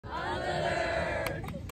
UNLITTER JUPITER, FL
Beach cleanup at Jupiter inlet in June 2033.